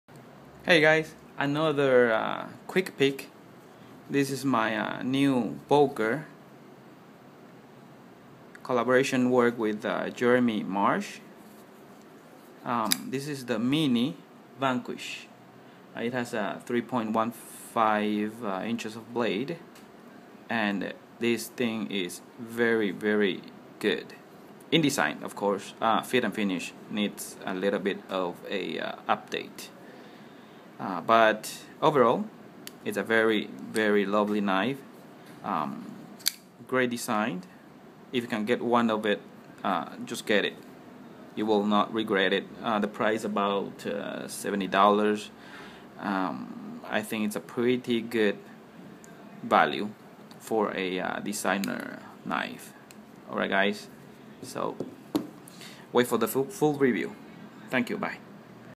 Boker Mini Vanquish Quikie
Just got it and I had to share!  Full length video coming up soon.